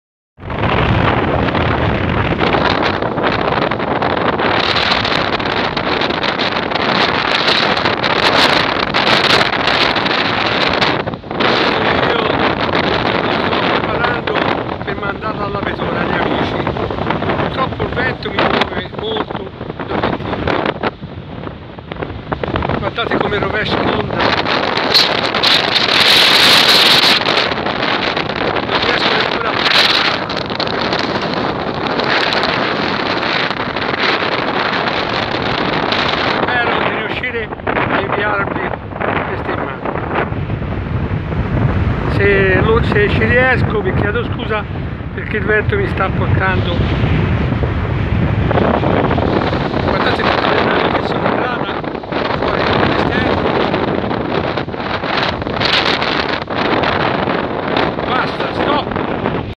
LIBECCIATA a LIVORNO
un meraviglioso mare di verde smeraldo sono titolare di molti video da me realizzati( 2600)con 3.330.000 visualizzazioni e 2OOO iscritti al mio canale